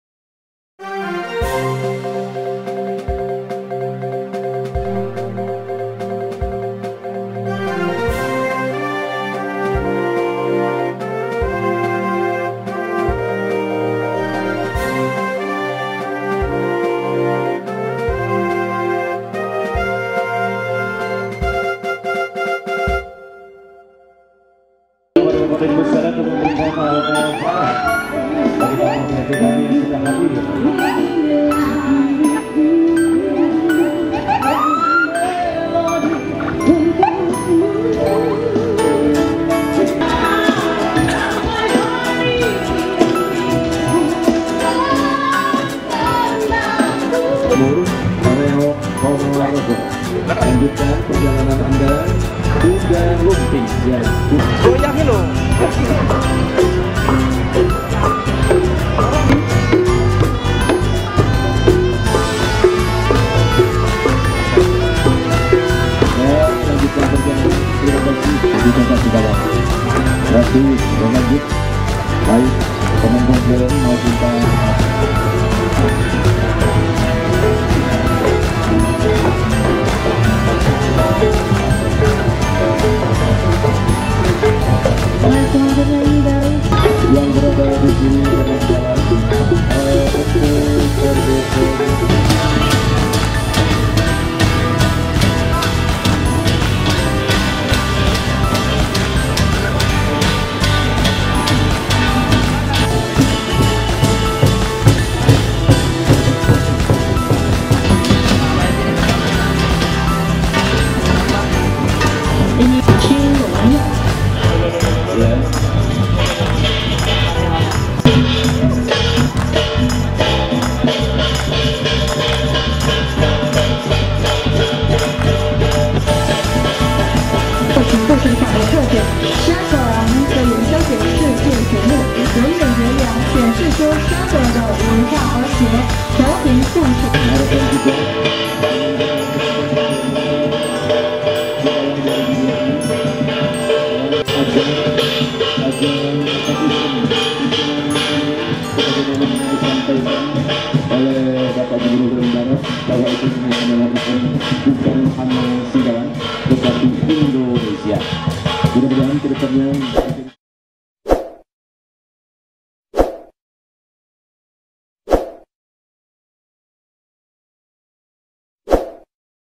Festival Cap Go Meh di Singkawang 2020
Tatung identik dengan perayaan Cap Go Meh di Singkawang. Kali ini jumlah Tatung yang ikut dalam festival cap Go Meh 2020 terus bertambah menjadi 847 yang merupakan jumlah tatung terbesar

Tatung adalah orang sakti yang dirasuki para dewa. Selain Tatung ada beragam atraksi multi etnis lainnya.